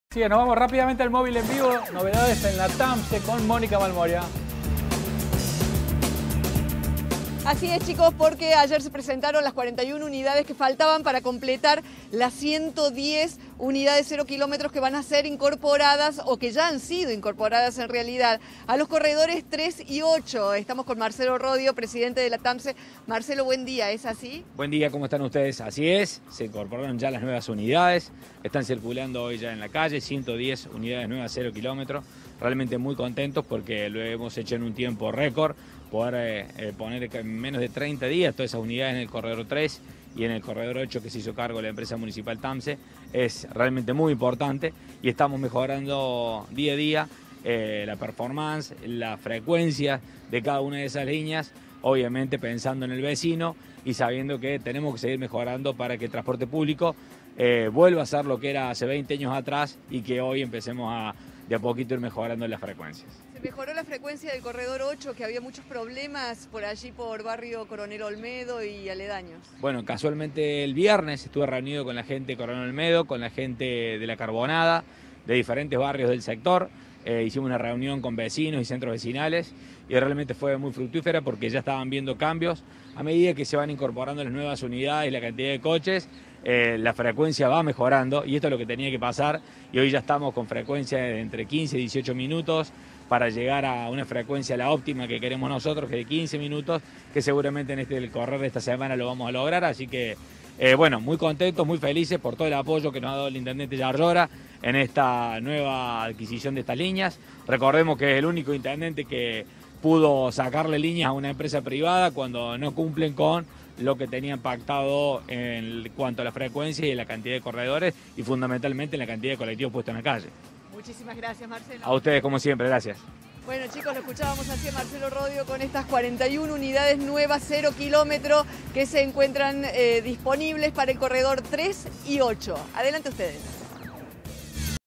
TAMSE pone en marcha 41 unidades 0KM restantes
A los 69 colectivos presentados el 30 de septiembre, hoy se suman otros 41. Totalizan 110 nuevos buses que recorren la ciudad.